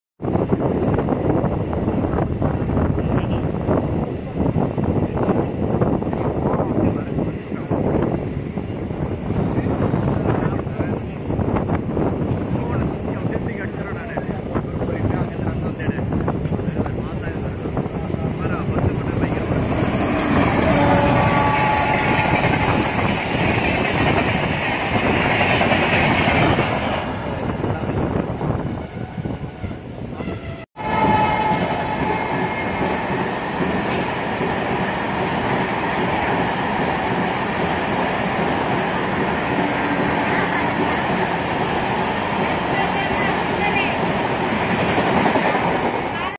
WAP-7 (RPM) rocks with SBC--MAS DOUBLE DECKER EXPRESS
WAP-7 Royapuram (RPM) rushes with daily running KSR BENGALURU (SBC) --- MGR CHENNAI CENTRAL (MAS) DOUBLE DECKER EXPRESS (SMALLEST TRAIN) only 10 coaches including 2 EOG with EPIC TRACK SOUNDS

Raise the volume & feel the track sounds of 10 coaches